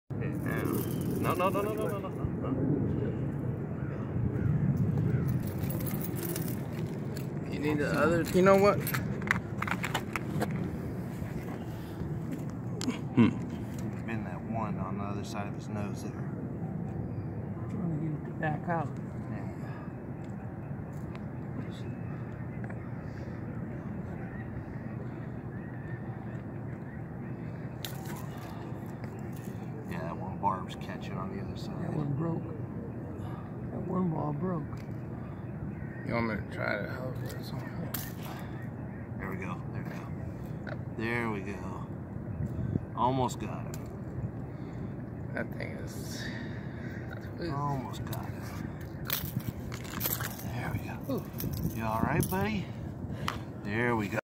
Saving a seagull with a hook in its beak with a Leatherman
The Leatherman tool really came  in handy